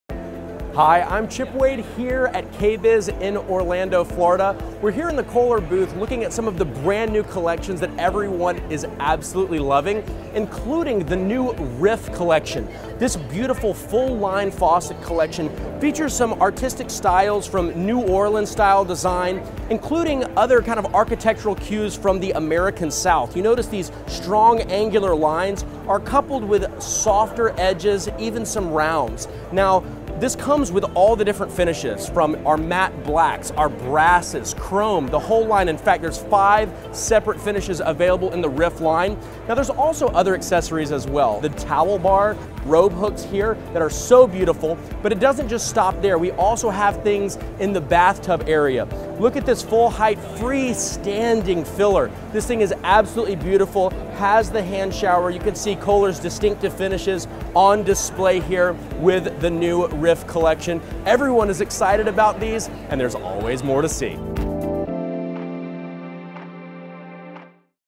Chip Wade Highlights the KOHLER’s Riff Faucet at KBIS 2022
Expert craftsman and television host Chip Wade discusses Kohler’s Riff faucet from the show floor of KBIS 2022.